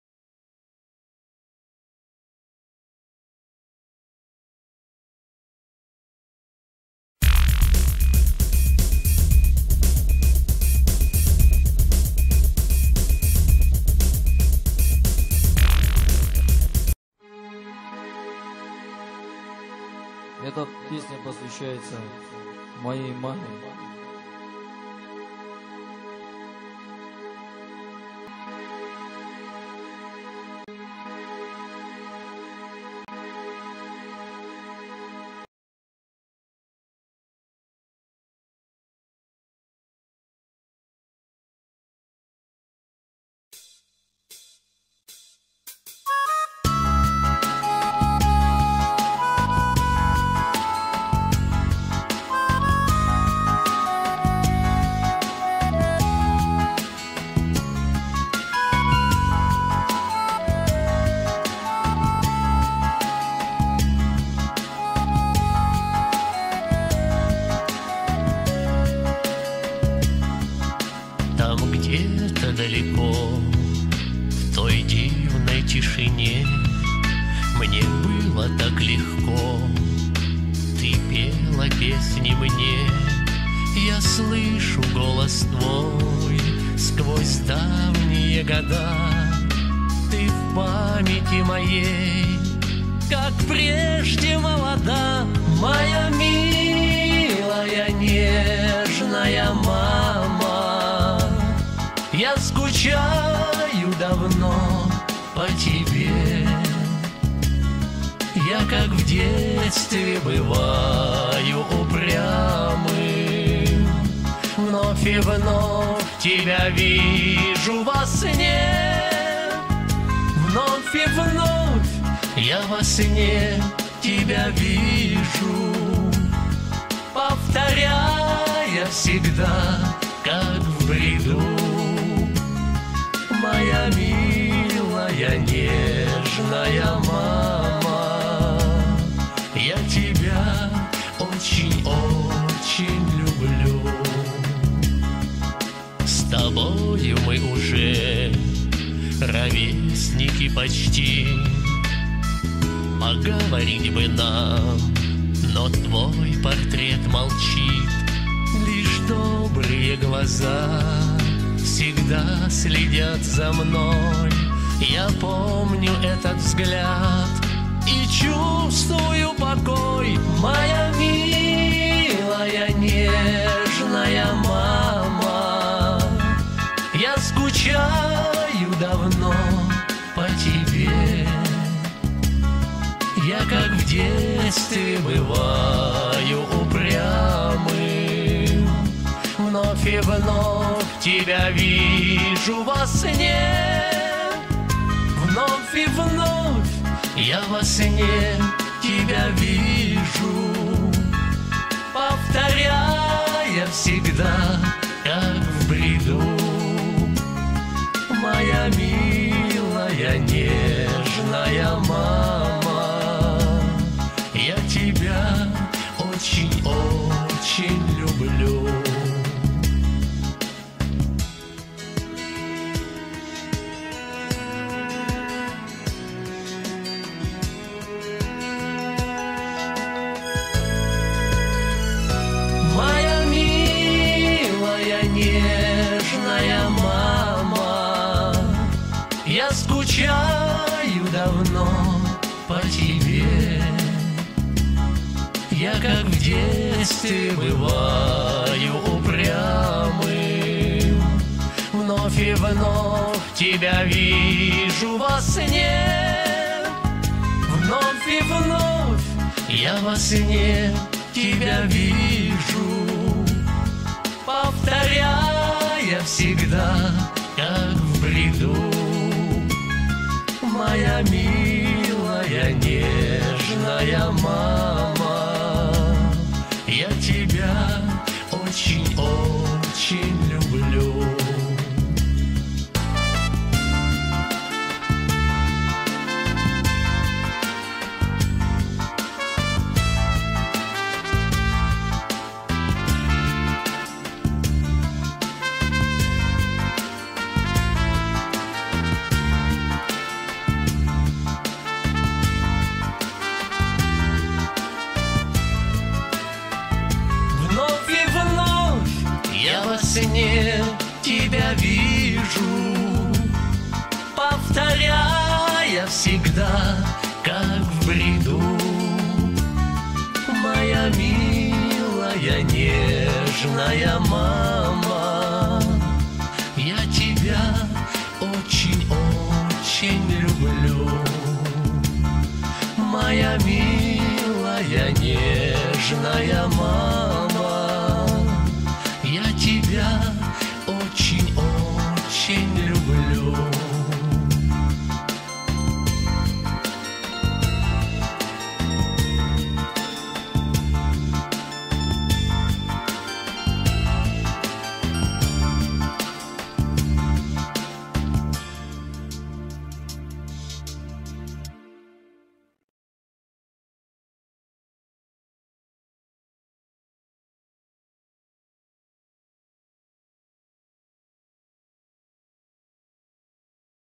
Моя Милая Нежная МАМА
Этот видео-ролик Посвящается ПАМЯТИ  моей дорогой МАМЕ